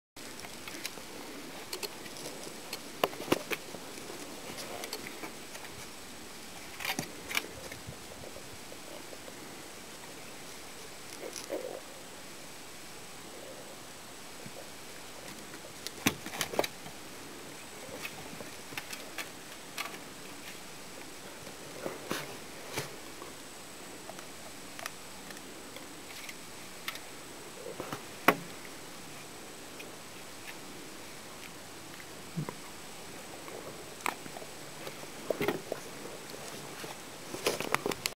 Macchia e Zorro
Macchia e Zorro giocano con le mele